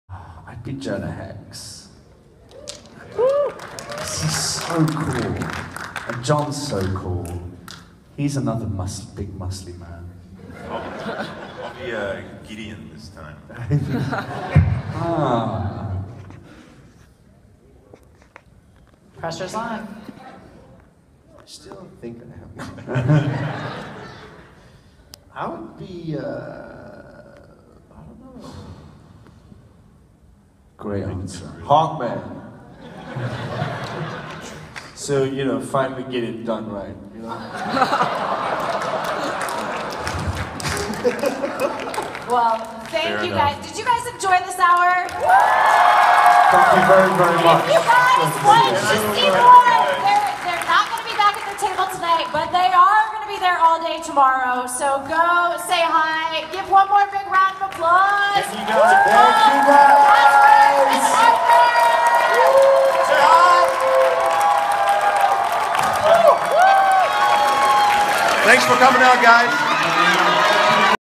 RICC 2016: Arthur Darvill, Falk Hentschel and Casper Crump on Playing Other Arrowverse Characters
RICC 2016: Arthur Darvill, Falk Hentschel and Casper Crump on Playing Other Characters

Three of the stars of Legends of Tomorrow discuss what other characters they would like to play on the show.

Don't forget make the Screen Avenger your first stop for movie and TV news, trailers and reviews on YouTube today!